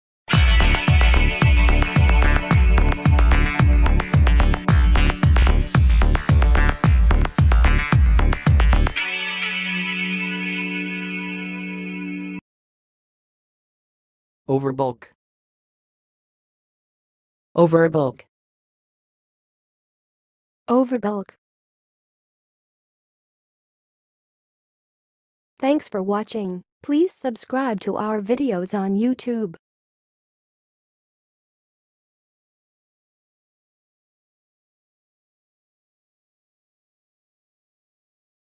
How to say "overbulk"! (High Quality Voices)
Watch in this video how to say and pronounce "overbulk"!